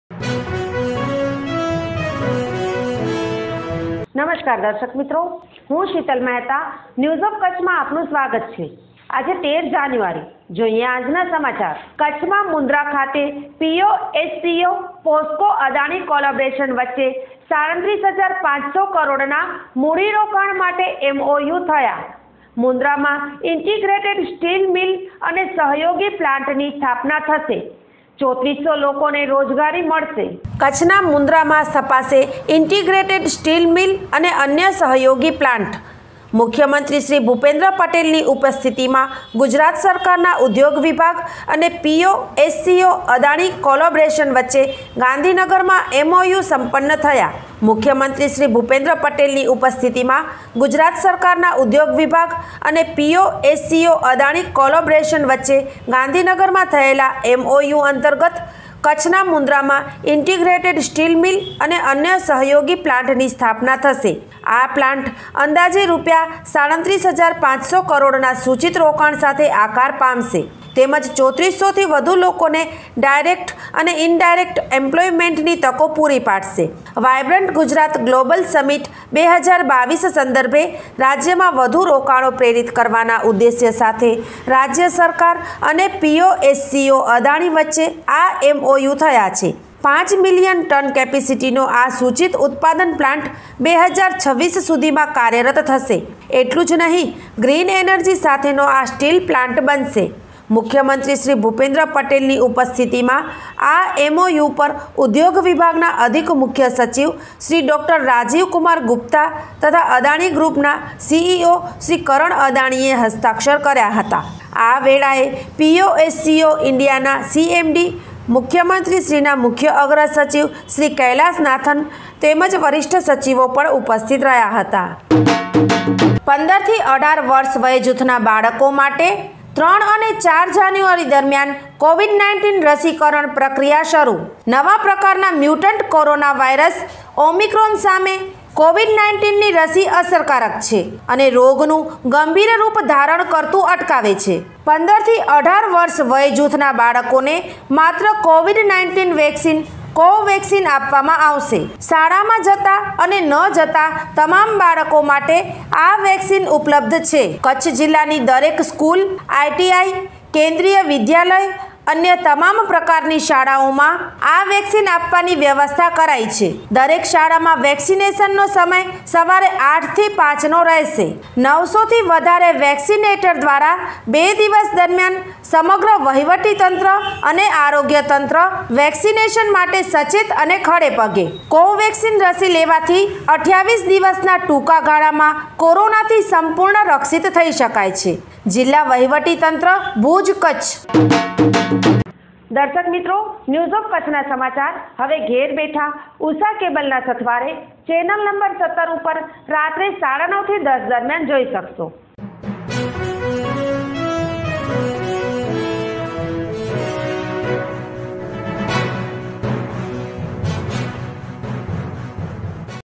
POSCO અદાણી કોલોબ્રેસન વરચે 37500 કરોડના MOU થયા
POSCO અદાણી કોલોબ્રેસન વરચે 37500 કરોડ ના MOU થયા મુન્દ્રામાં ઈન્ટિગ્રેટેડ સ્ટીલ અને સહયોગી પ્લાન્ટની સ્થાપના થશે 3400 લોકોને રોજગારી મળશે🌹